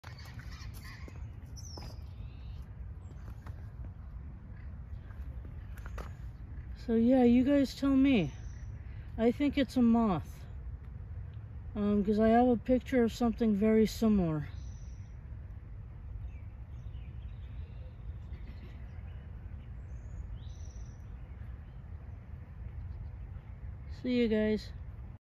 I found out what it’s called
Hadenacapsicola or a Yellow Striped Army Worm, and it’s a moth.  What do you think?